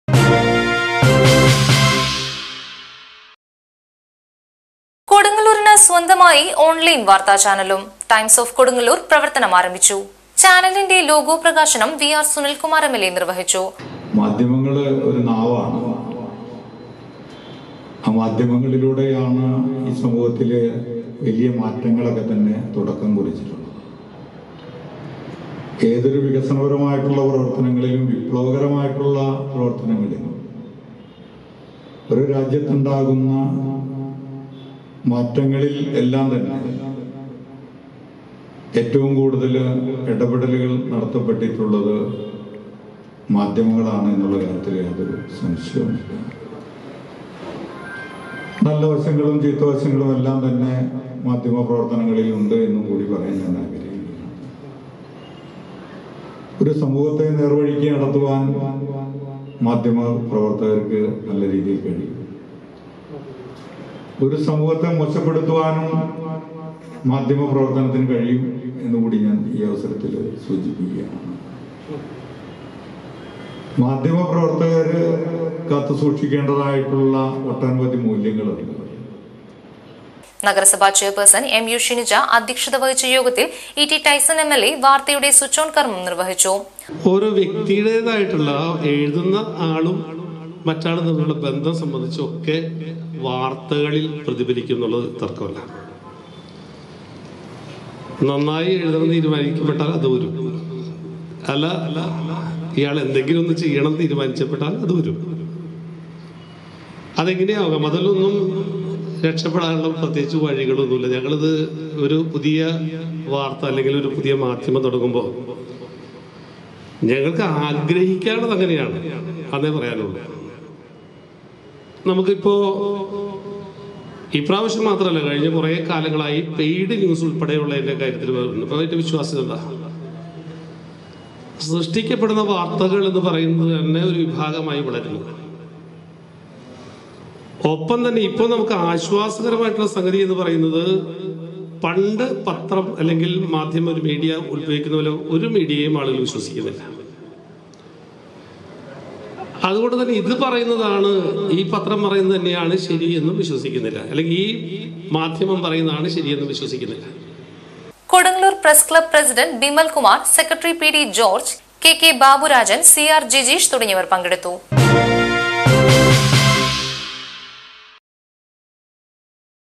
CRANGANORE NEWS കൊടുങ്ങല്ലൂരിന് സ്വന്തമായി ഓൺലൈൻ വാർത്താ ചാനലും, ടൈംസ് ഓഫ് കൊടുങ്ങല്ലൂർ പ്രവർത്തനം
കൊടുങ്ങല്ലൂരിന് സ്വന്തമായി ഓൺലൈൻ വാർത്താ ചാനലും, ടൈംസ് ഓഫ് കൊടുങ്ങല്ലൂർ പ്രവർത്തനം ആരംഭിച്ചു